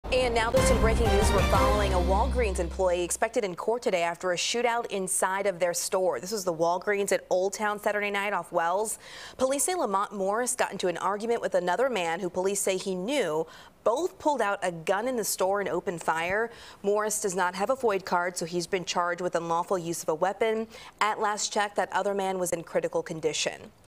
Chicago man charged in shootout inside Near North Side Walgreen's store
Police said Lamont Morris, 26, who was an employee at the store, was in an argument with a 32-year-old man inside before the two pulled out guns and exchanged gunfire.